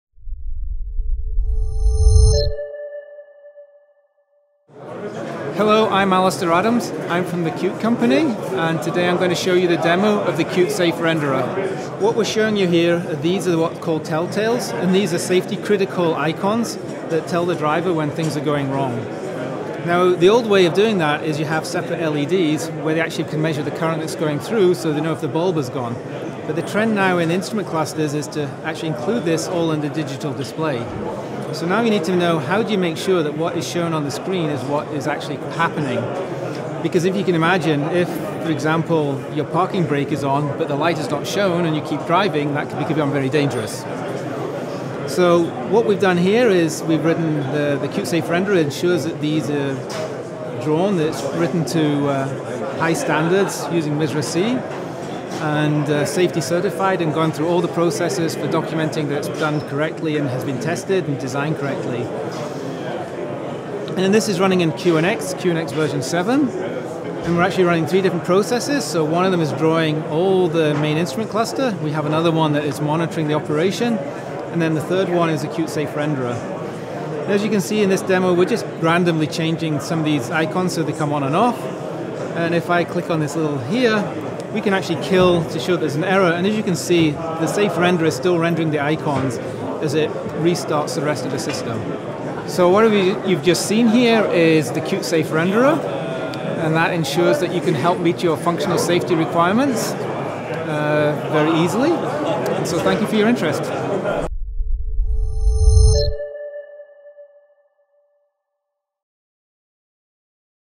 Qt Safe Renderer
With Qt Safe Renderer you are able to separate the functional safety critical parts of your software to prevent unacceptable harm to people by lowering the impact of failures.